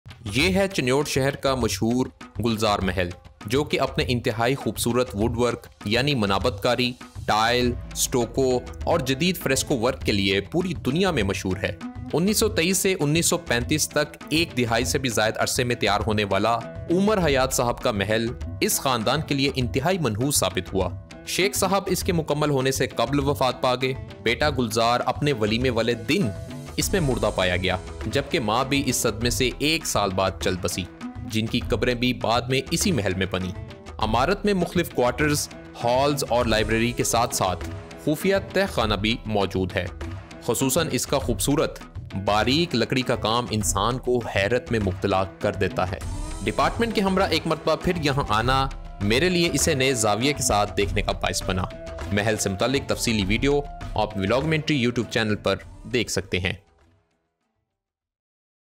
Exploring The Historic Beauty Of Chiniot: Omar Hayat Mahal 1923
Omar Hayat Mahal (Urdu: عمر حیات محل), also spelt Umer Hayat Mahal, and alternatively known as Gulzar Manzil, is an early 20th-century wooden haveli mansion in Chiniot, Pakistan.
The mansion was started in 1923, and was completed by 1935. The mansion, originally 5 stories tall, was built by Sheikh Omar Hayat – a Chinioti businessman who had made his fortune in Calcutta. The edifice displays outstanding examples of Chiniot's local woodworking style, and has been described as an "ornament" of a building on account of its lavishly decorated interior